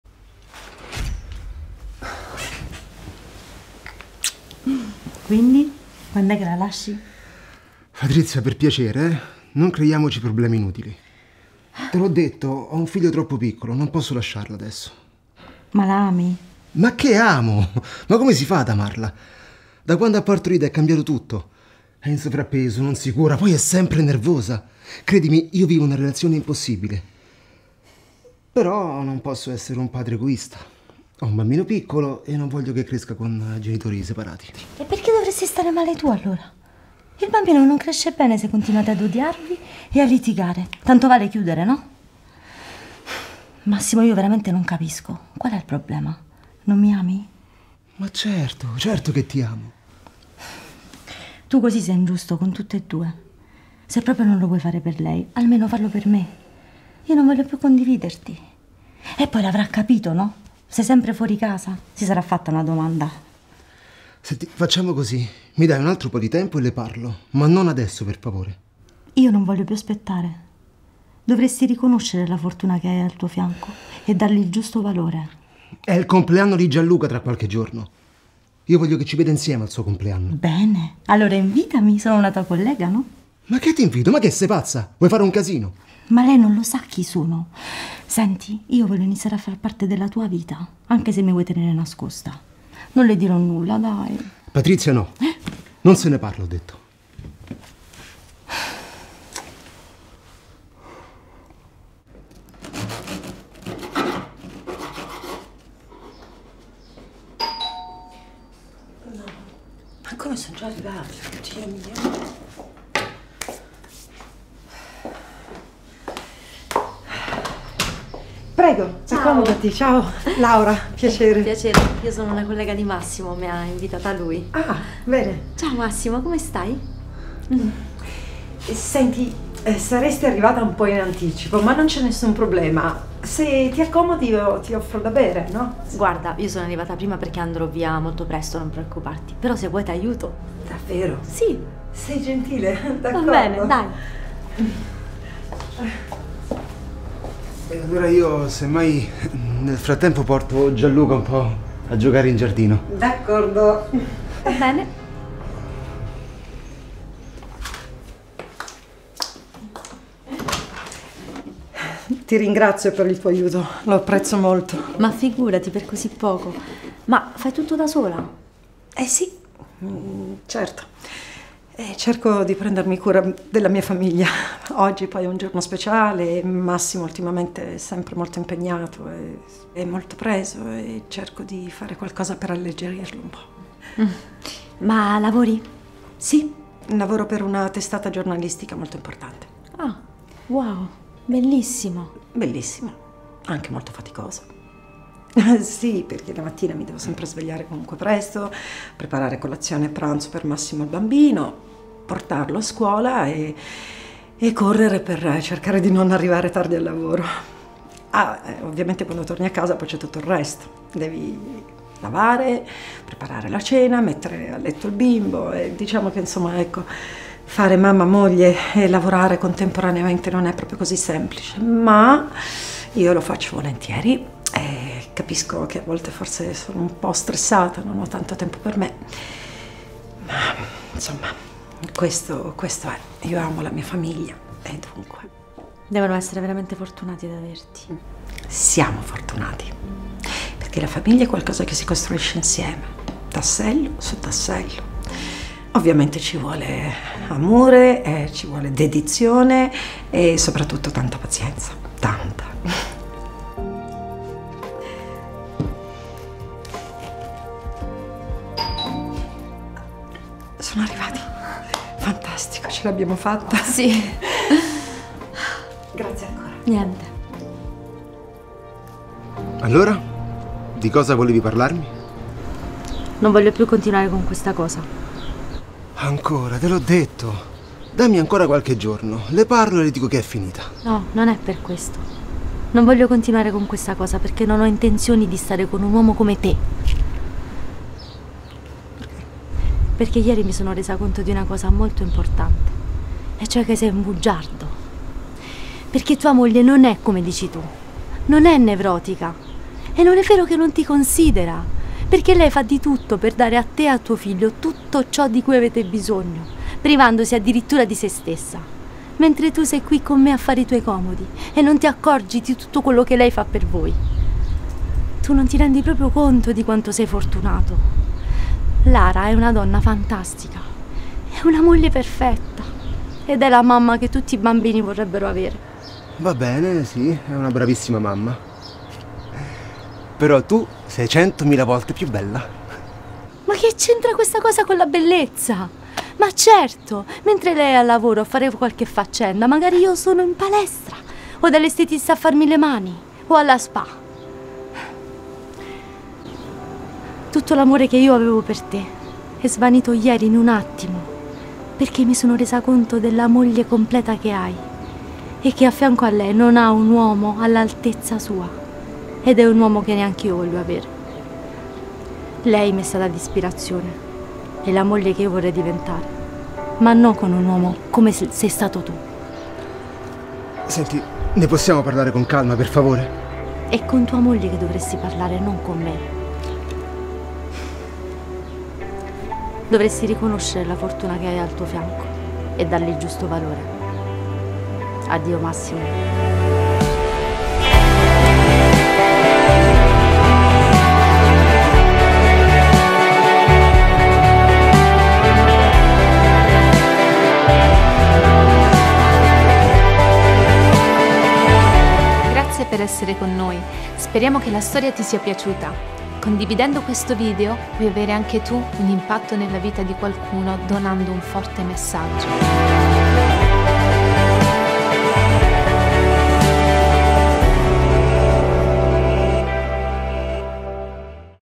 Vuole costringere l'amante a divorziare, accadrà qualcosa di inaspettato
Storia d'impatto 🔥
Simona Rubino ha un messaggio per te : Dovremmo imparare a dare valore a ciò che abbiamo al nostro fianco... A volte non ci rendiamo conto di ciò che una persona fa per noi o tendiamo a darlo per scontato... e ciò può portarci a commettere errori, a volte irreparabili... quando qualcuno ci ama, dovremmo riconoscerlo ogni giorno e ringraziare della grande opportunità di avere una persona così al nostro fianco che ci supporta e ci ama, nonostante tutto... è questa la vera bellezza... il vero amore che una persona è in grado di donarci...
Segui Story Impact Italia per più storie 👀

Crediti:
Attori:
Cristiano D'alterio
Claudia Dezio
Daniela Fazzolari

Prodotto da:
Story Impact Italia